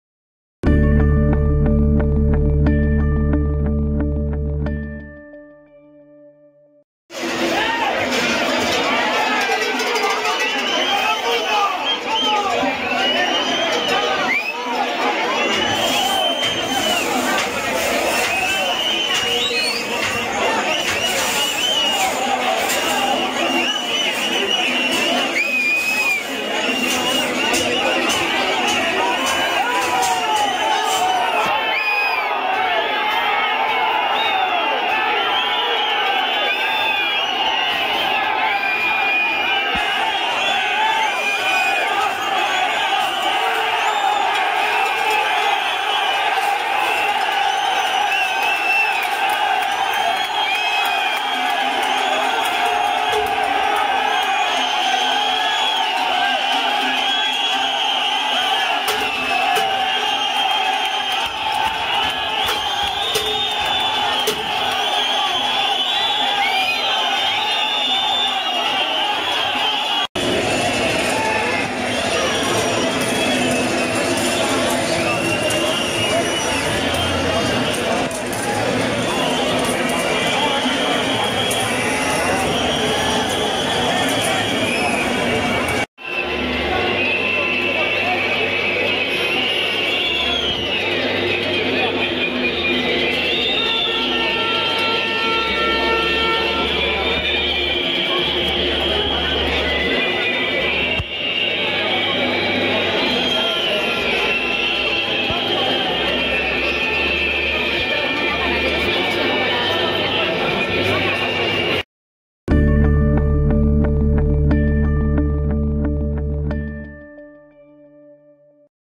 Antidisturbios tienen que intervenir ante las protestas de los ganaderos en Salamanca
Las protestas de los ganaderos ante la Delegación Territorial de la Junta llevan a intervenir a la Policía Nacional